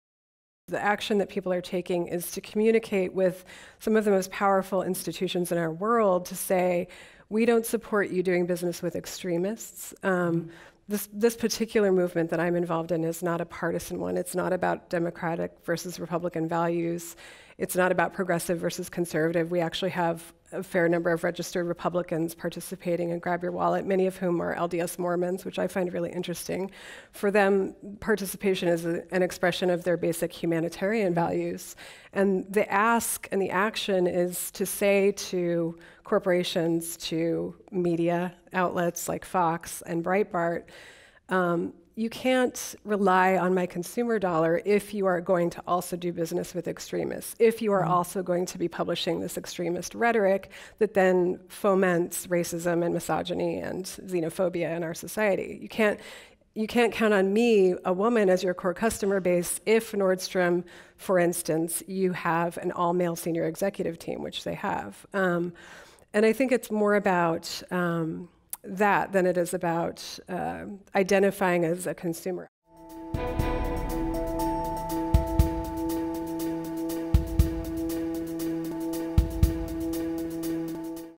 Shannon Coulter: Grab Your Wallet
GrabYourWallet founder Shannon Coulter explains how speaking with your money isn't a partisan issue.